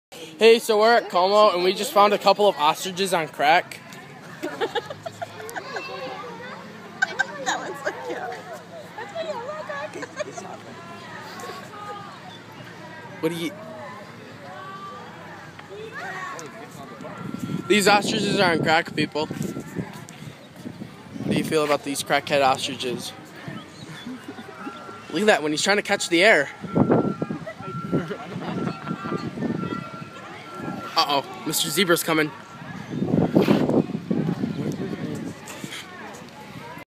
Ostriches on crack!
I was at Como Zoo in Minnesota with some friends, and we found these ostriches. They had been acting weird for quite awhile, so I had come to the assumption that, they were indeed in crack.
  Control your animals Como!